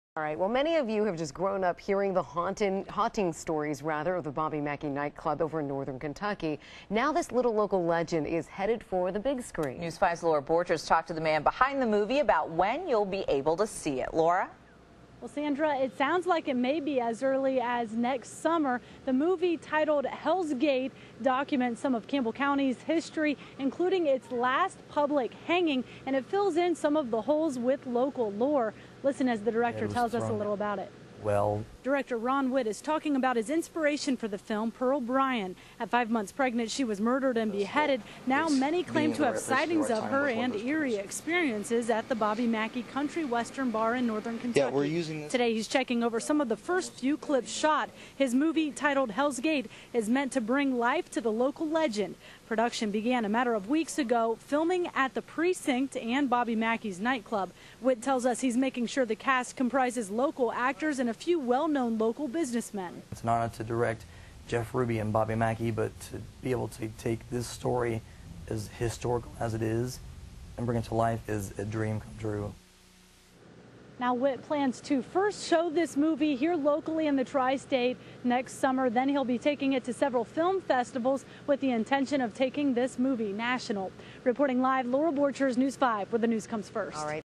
Haunted Night Club?
A Campbell County, Kentucky night club will soon be featured in a feature film.